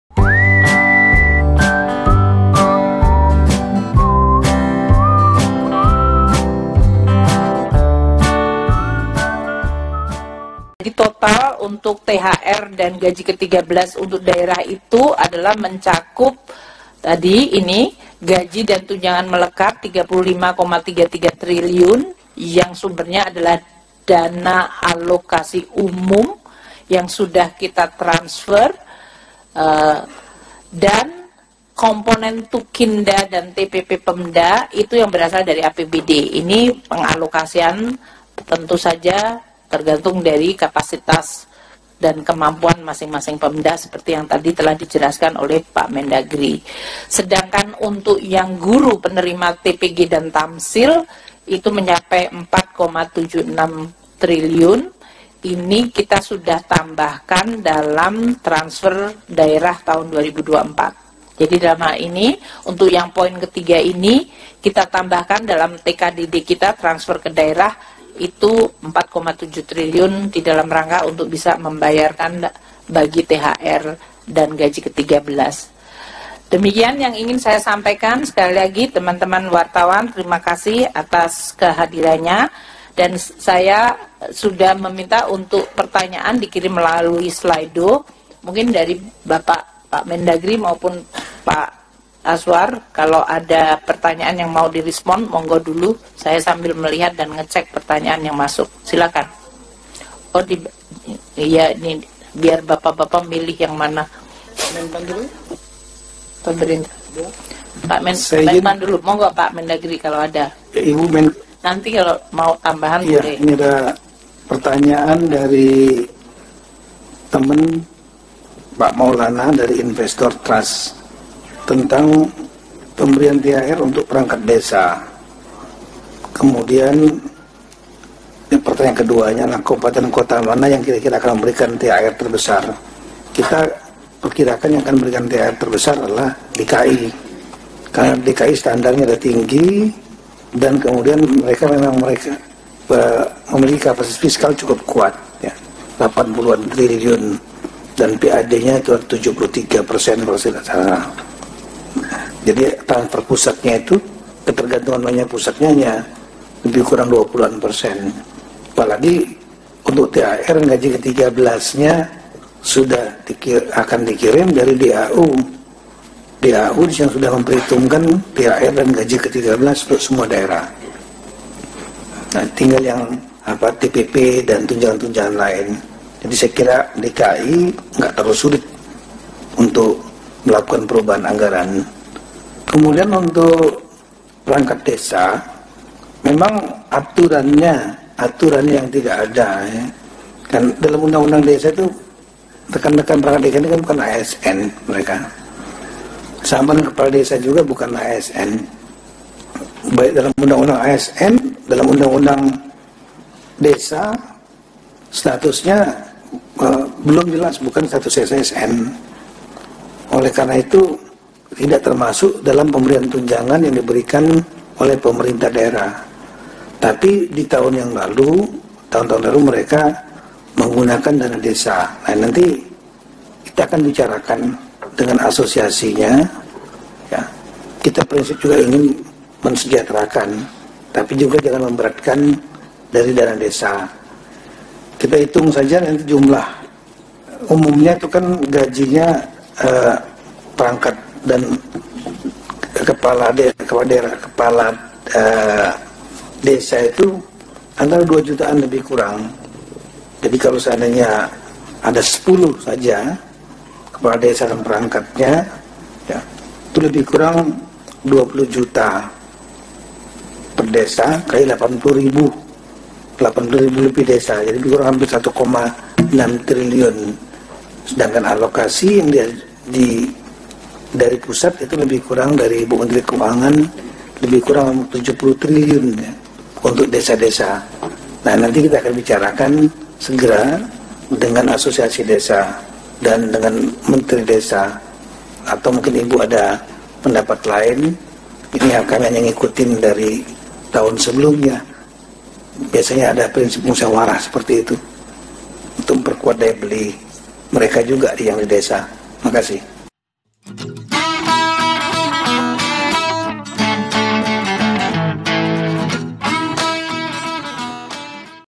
Hore kabar gembira pidato menteri keuangan dana tambahan 100 persen tunjangan sertifikasi guru 2024
Ini adalah kabar gembira dari pemerintah yang diumumkan oleh menteri keuangan perihal tambahan 100 persen tunjangan sertifikasi guru dalam gaji 13 tahun 2024. Pemerintah telah menetapkan peraturan pemerintah nomor 14 tahun 2024 tentang THR dan gaji 13. Dalam peraturan tersebut tercantum pasal yang menjelaskan bahwa guru dapat diberikan 100 persen tunjangan sertifikasi dalam 1 bulan dalam THR dan gaji 13.